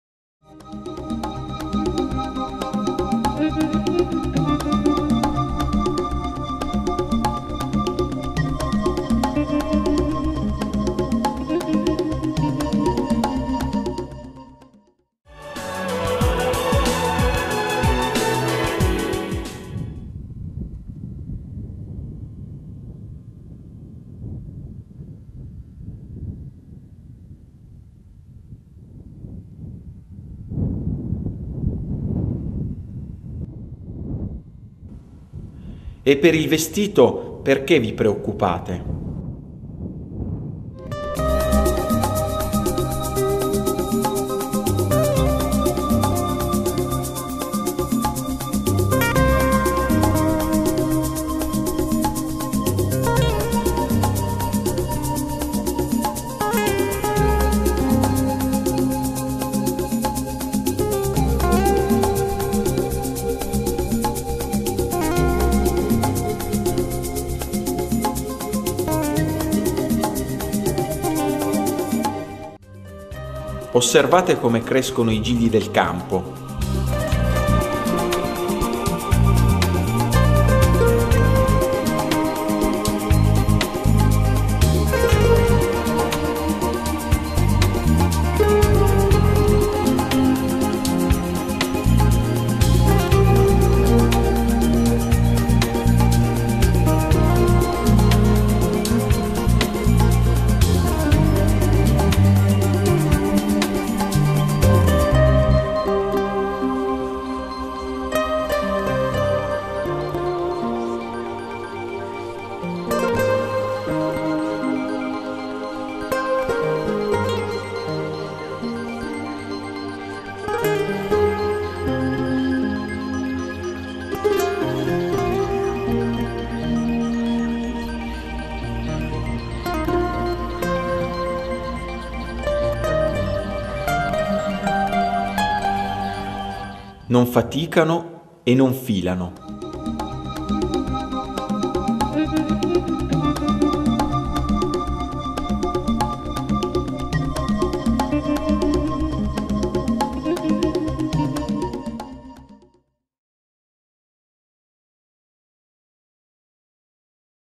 BubbleBible I GIGLI DEL CAMPO
BUBBLE BIBLE: I GIGLI DEL CAMPO
BIBBIA (MATTEO 6,28): "... E PERCHE' VI AFFANNATE PER IL VESTITO? OSSERVATE COME CRESCONO I GIGLI DEL CAMPO...NON FATICANO  E NON FILANO...".
BIBLE (MATTHEW 6,28): "... AND WHY WORRY ABOUT CLOTHING? THINK OF THE FLOWERS GROWING IN THE FIELDS; THEY NEVER HAVE TO WORK OR SPIN...".
GESU' INVITA A PREGARE E A CHIEDERE A DIO-NOSTRO PADRE- IL NECESSARIO PER VIVERE, QUELLO DI CUI ABBIAMO BISOGNO OGNI GIORNO. ABBIAMO BISOGNO DI POCO...AI GIGLI CHE CRESCONO SPONTANEAMENTE NEI CAMPI BASTANO LA LUCE DEL SOLE E L'ARIA...
LE IMMAGINI DI QUESTO VIDEO SNO STATE REGISTRATE NEL DESERTO DELLA GIORDANIA.